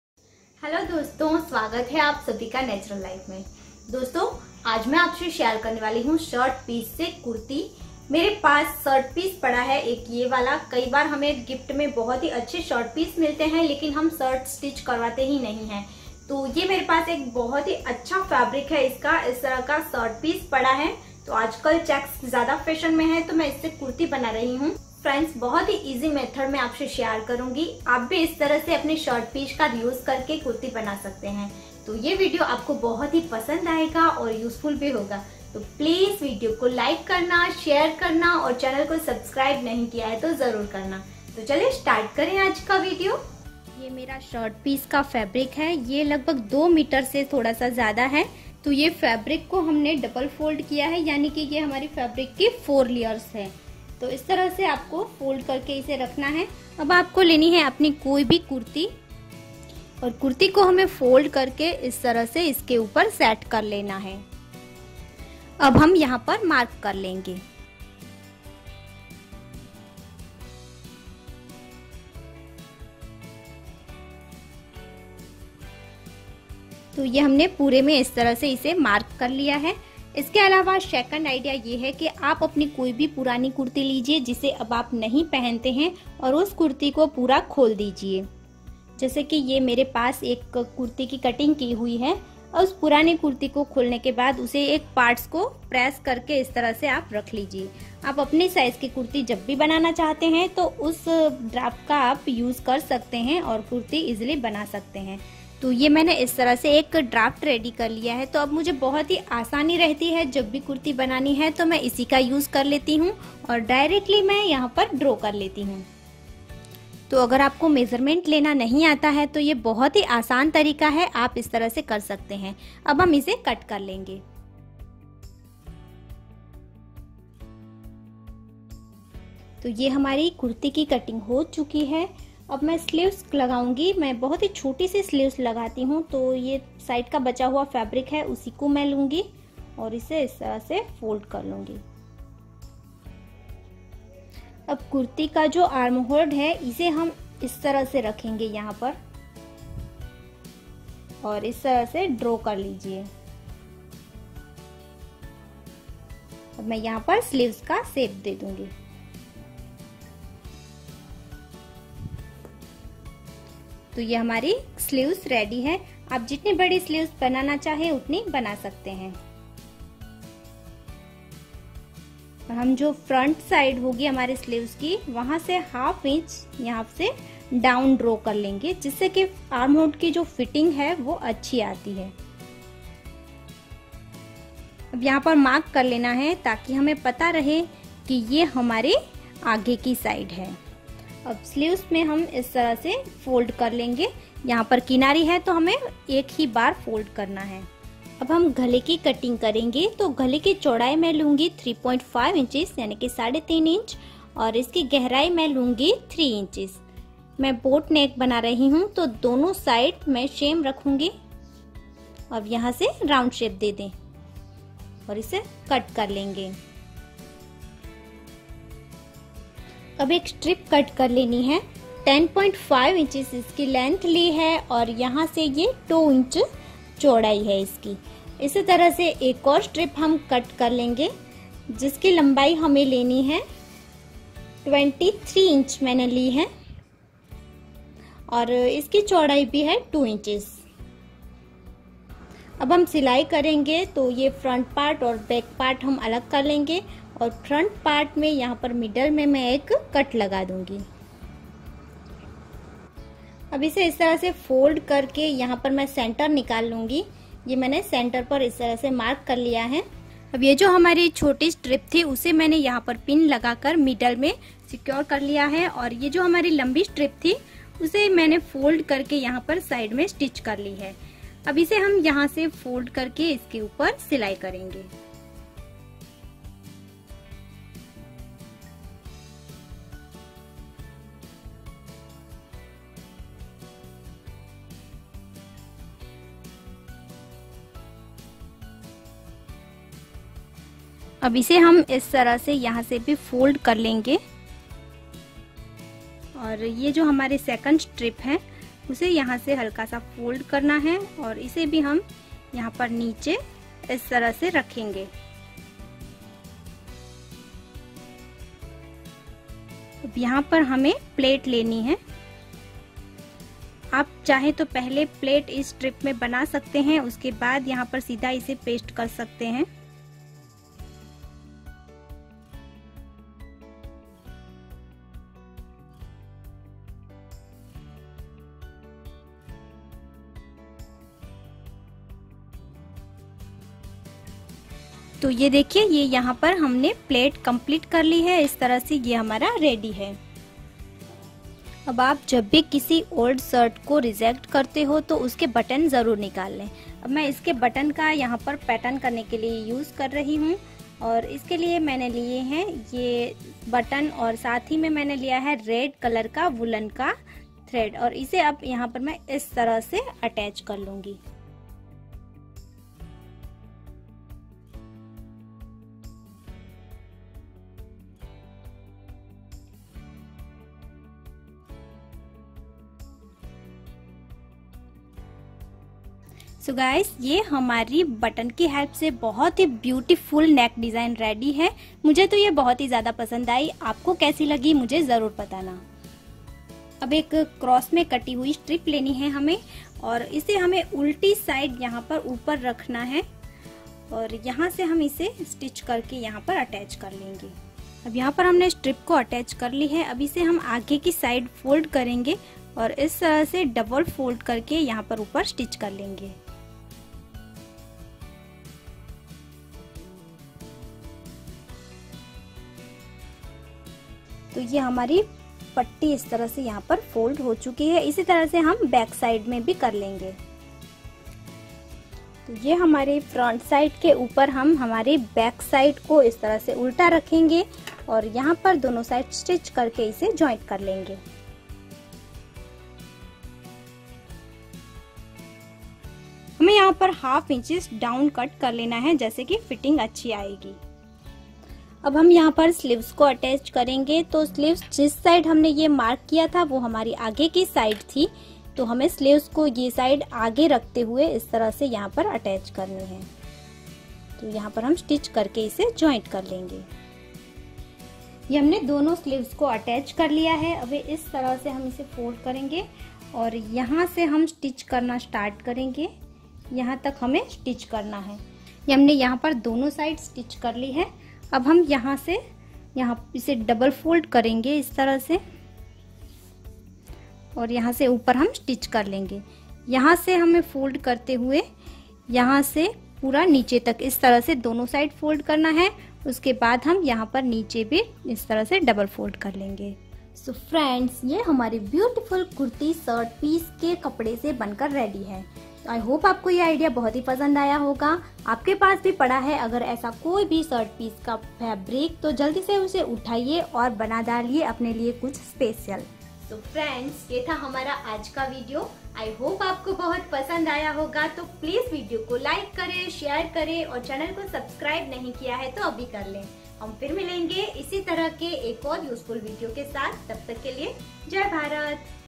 शर्ट पीस का ऐसा इस्तेमाल आपने पहले कभी नहीं किया होगा / How to make kurti from shirt piece
शर्ट पीस का यह इस्तेमाल आपको जरूर पसंद आएंगा
 How to make Beautiful kurti from shirt piece